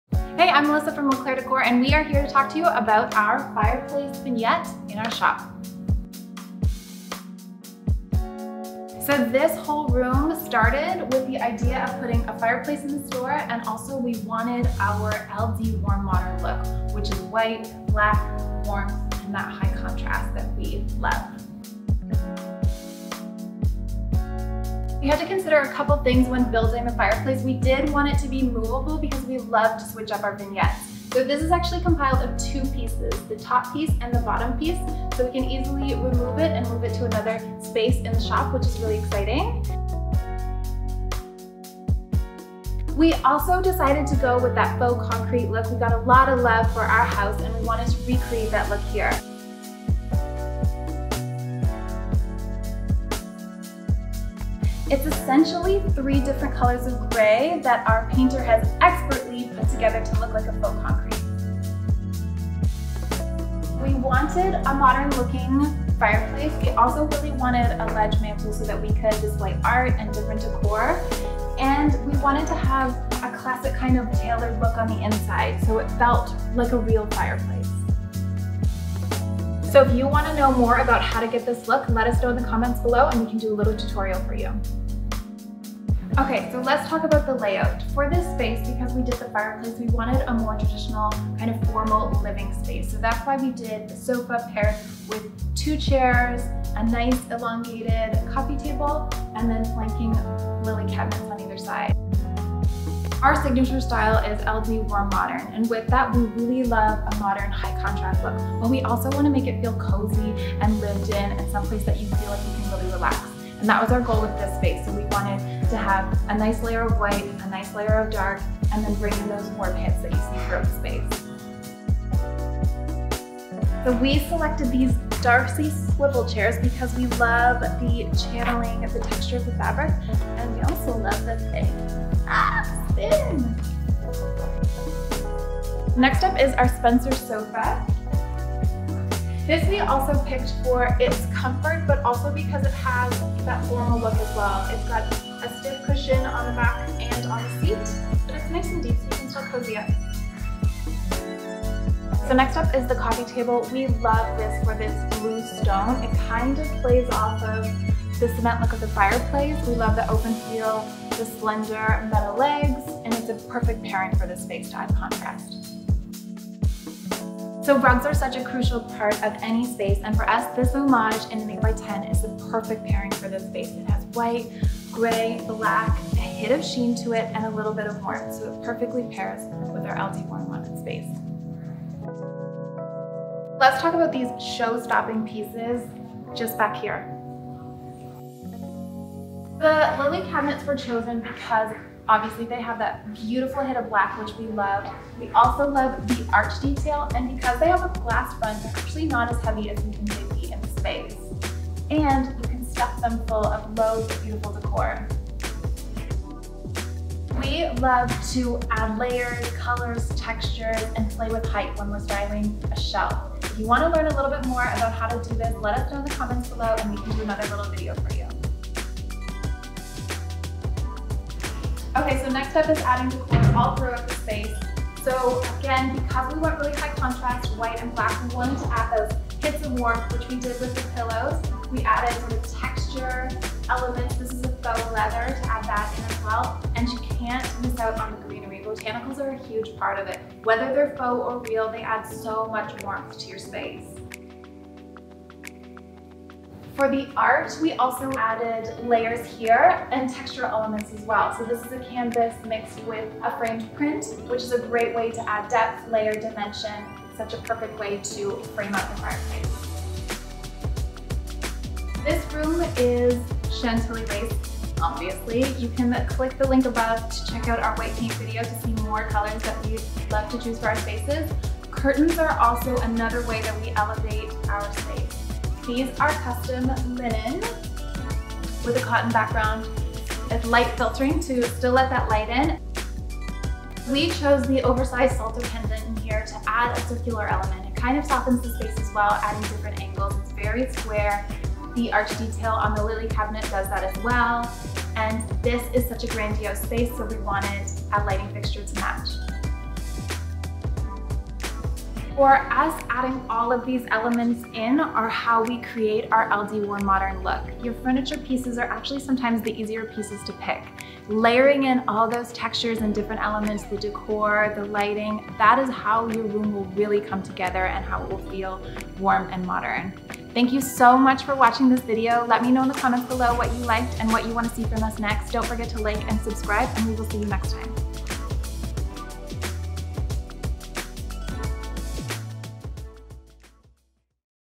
The Making Of: LD Shoppe Fireplace Living Room
We're so excited to take you behind the scenes of how we created this much asked about fireplace vignette! And yes - we're letting you know exactly how we achieved a plaster finish! We're diving into where the inspiration came from, and how we create our warm modern spaces.